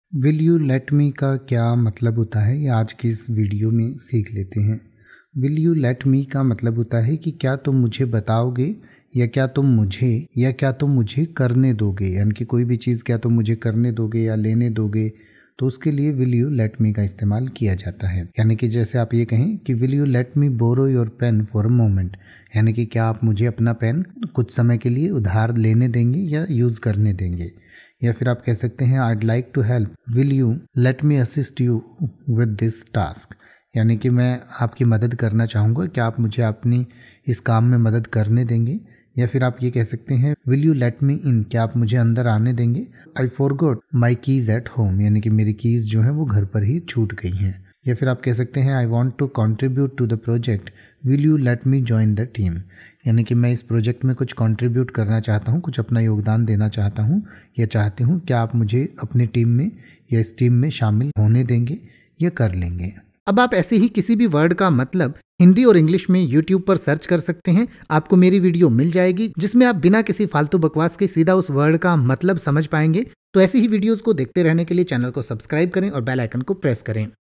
Will you let me meaning in Hindi | Will you let me ka matlab kya hota hai | English to hindi
इस वीडियो में आप Will you let me का हिंदी में मतलब समझेंगे और साथ में Will you let me का pronunciation भी सीखेंगें यानि की आप Will you let me के अर्थ के साथ साथ ये भी सीखेंगें की Will you let me को कैसे बोले या Will you let me को बोलने का सही तरीका क्या है |

तो बस एक मिनट में सीखिए Will you let me को|
Let’s learn Hindi Meaning of Will you let me in detail.

Will you let me का हिंदी में अर्थ होता है
क्या तुम मुझे बताओगे, क्या तुम मुझे, क्या तुम मुझे करने दोगे

Will you let me borrow your pen for a moment?
I'd like to help. Will you let me assist you with that task?
Will you let me in? I forgot my keys at home.
I want to contribute to the project. Will you let me join the team?

Will you let me को इन उदाहरणों से और अच्छे से समझ सकते हैं